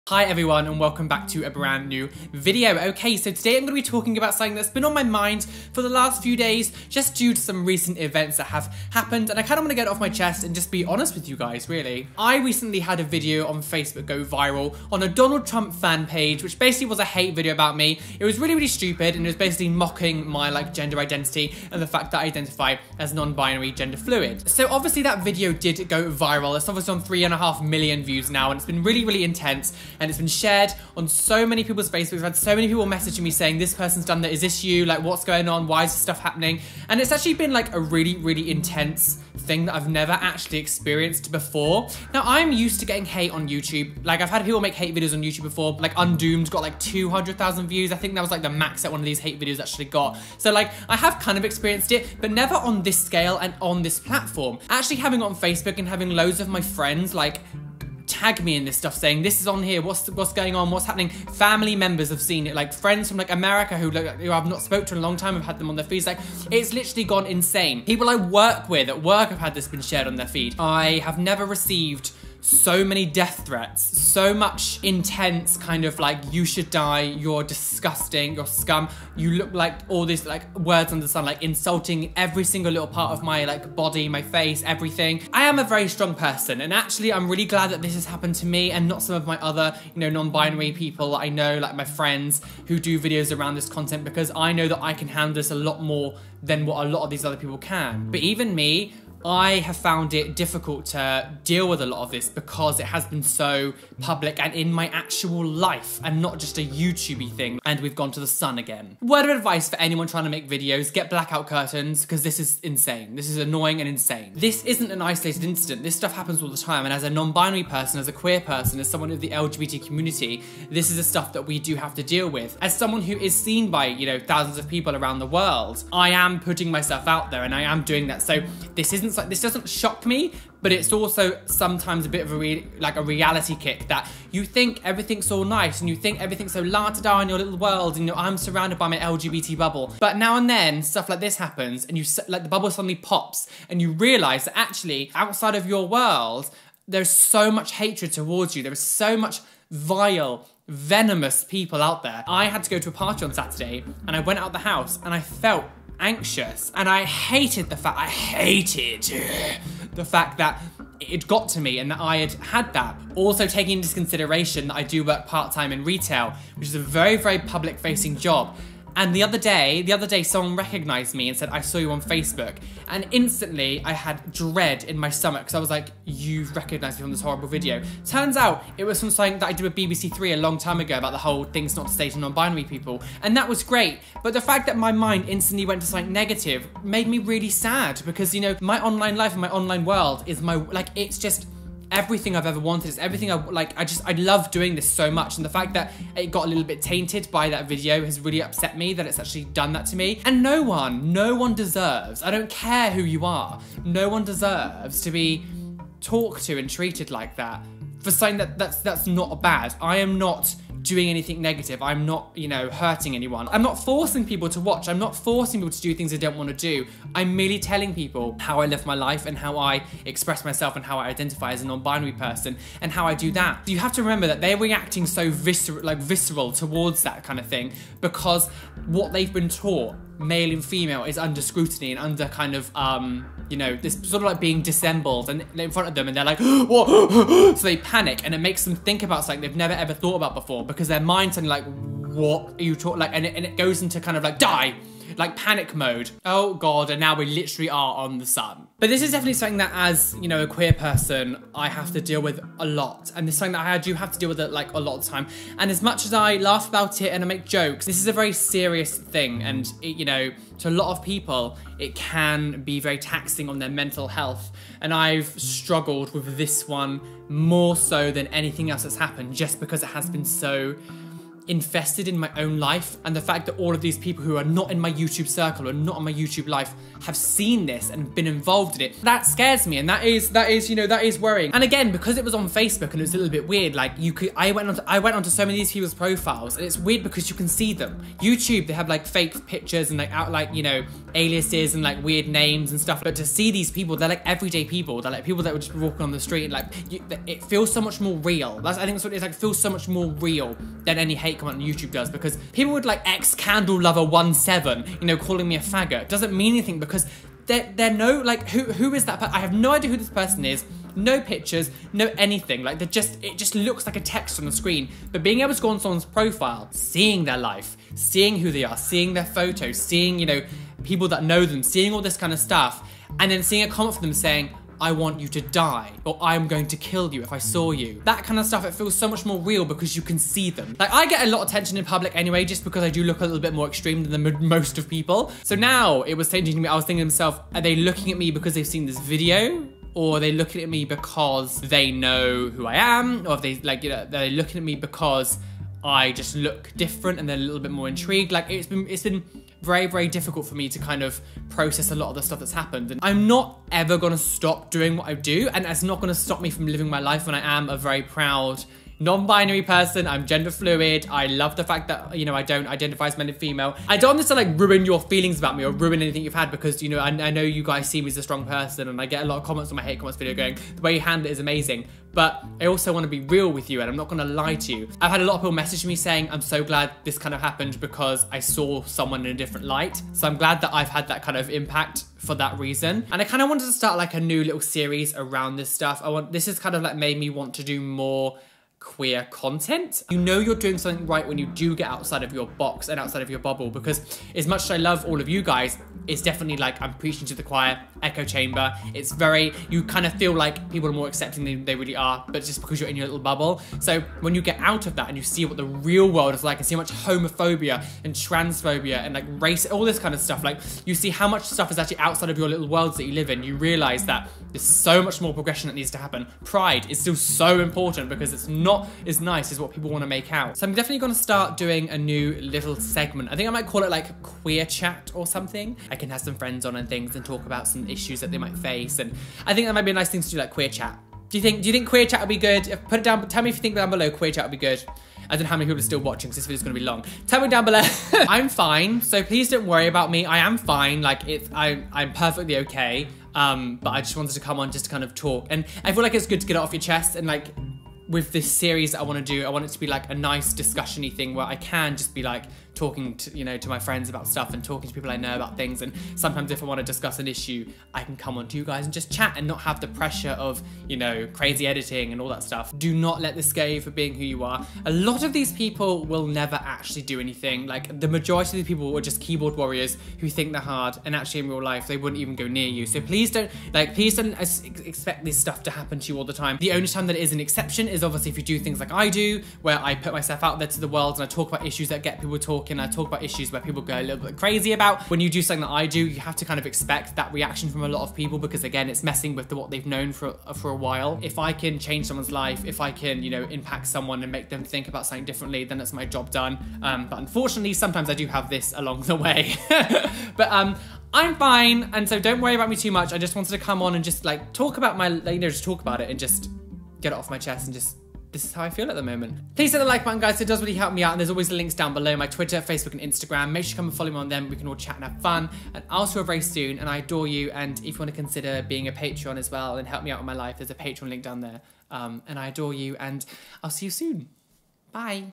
Death Threats For Being Non Binary | Queer Chat | Roly
Death Threats For Being Non-Binary! Queer Chat: Being Gender Fluid is great, but it also comes with negatives. After a Viral Hate Video went out agist me I have had More death threats than ever before! Being a Non-Binary Gender causes such a horrible reaction from some people and in this new series Queer Chat, I wish to discuss everything LGBTQ+ and Queer Topics.
ROLY MERCH (Shirts, Tanks & Hats)